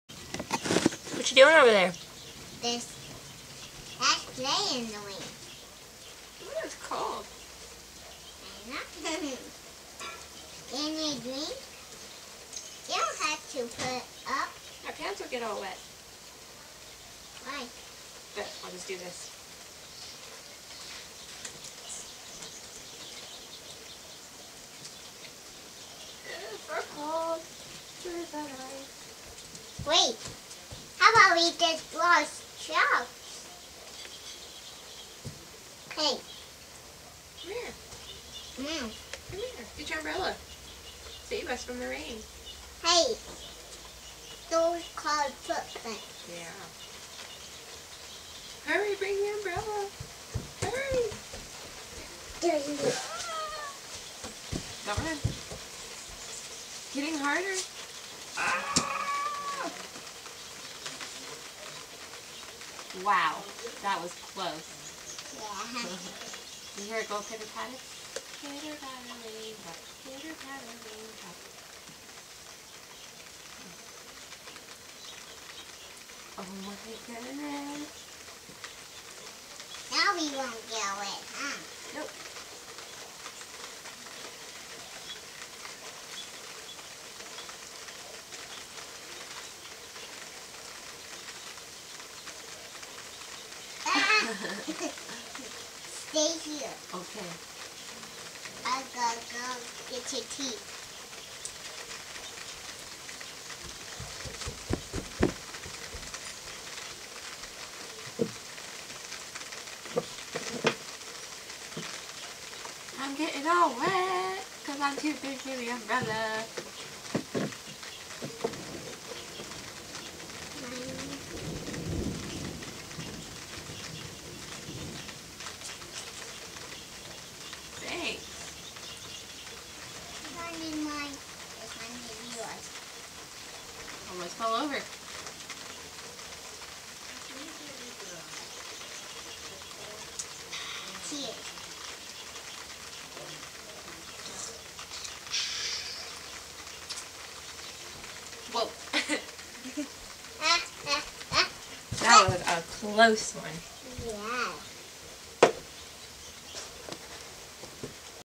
Playing in the rain
This is a video of my daughter and I just hanging out in the rain on my day off. Since I was little I have always loved to play in the rain and snow. I used to blow bubbles in the rain in my backyard when I was little and then once the grass got soapy enough I would play in the grass like a slip n slide. I hope you all enjoy our fun little clip.